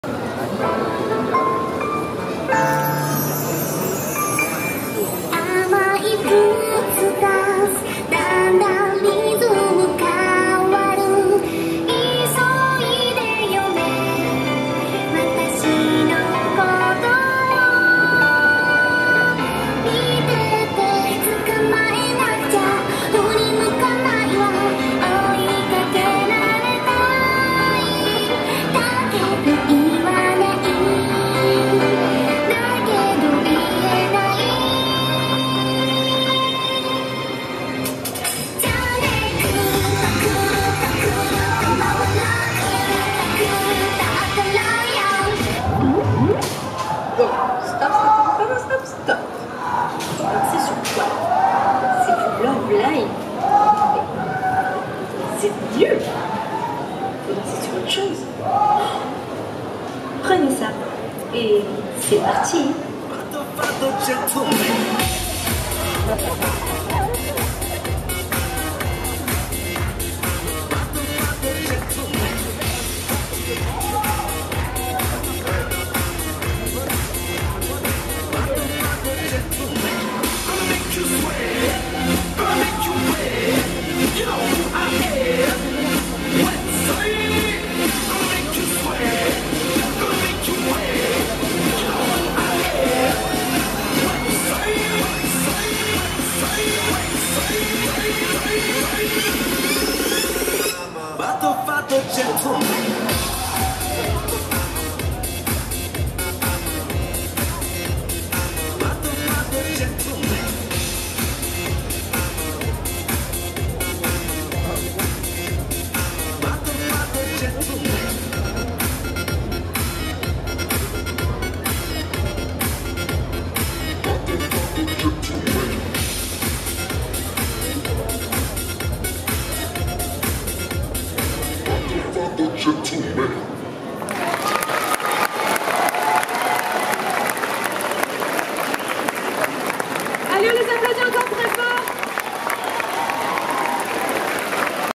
Manga Mania 2015 - Montelimar - Concours Cosplay - 01 - cross over
Crossover Love Live! School Idol Project et Kill la Kill

Special thanks
* To the Aoi Sora Cosplay association for the Cosplay management.
* The Festival team.
* The cosplayers.

Remerciements spéciaux:
* L'association Aoi Sora Cosplay (section lyonnaise) pour l'organisation du Cosplay.
* L'équipe du festival.
* Les Cosplayeurs.
* Le public de la convention.